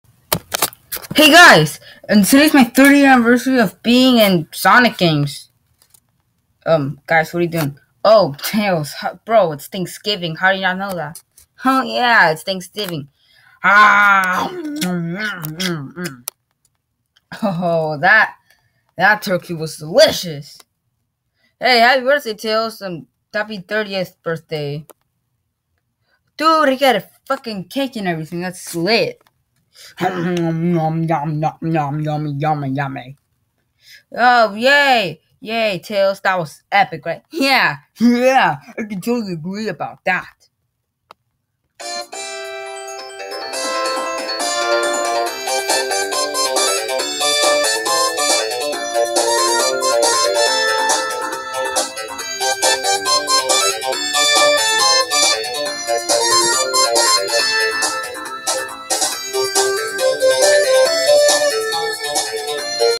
Tails 30th birthday
its the 30 year anniversary of tails being in sonic games